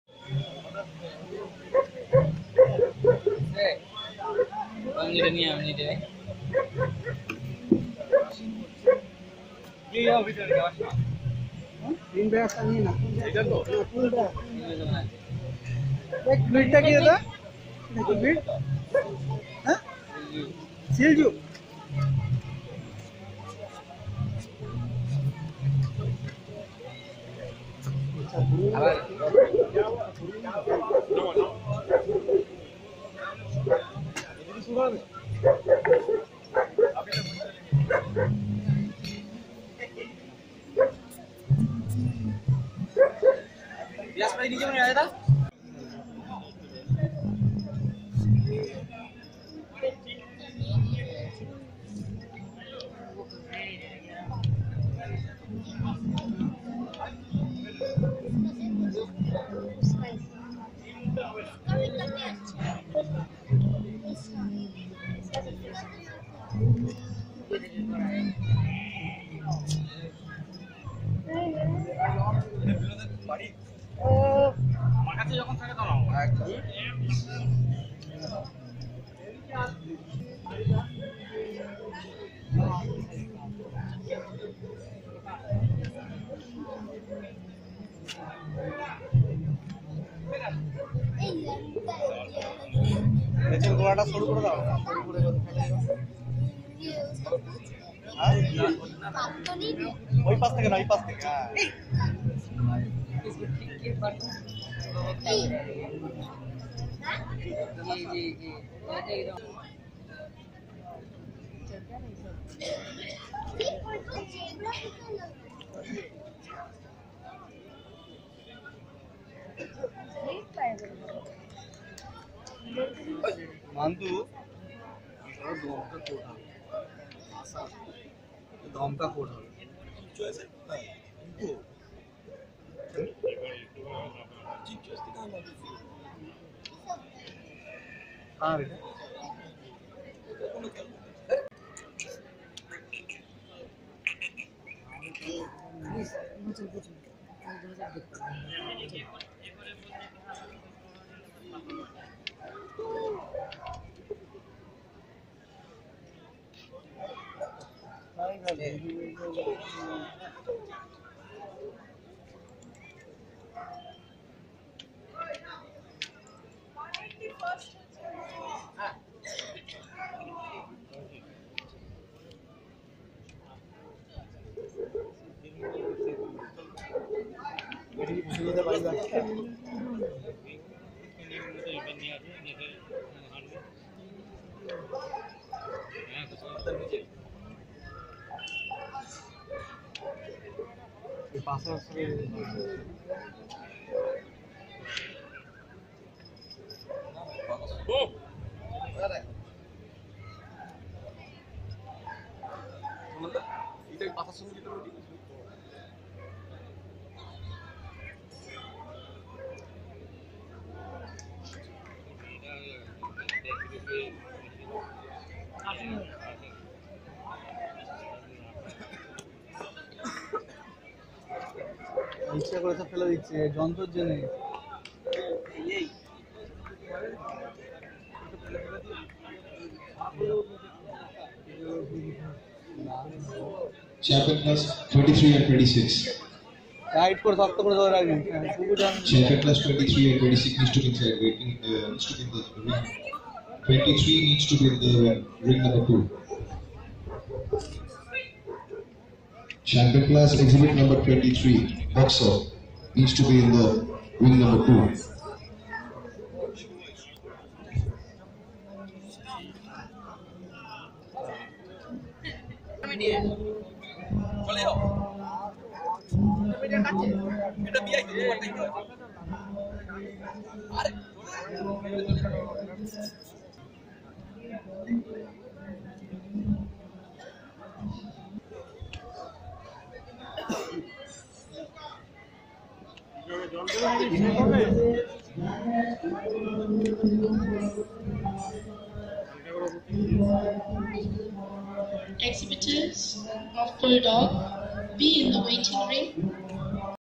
Beauty Queen Dog Lhasa Apso l Lhasa Apso in the Ring l Lhasa Apso in City of Joy Dog Show
The Lhasa Apso is a non-sporting dog breed originating in Tibet. It was bred as an interior sentinel in the Buddhist monasteries, to alert the monks to any intruders who entered.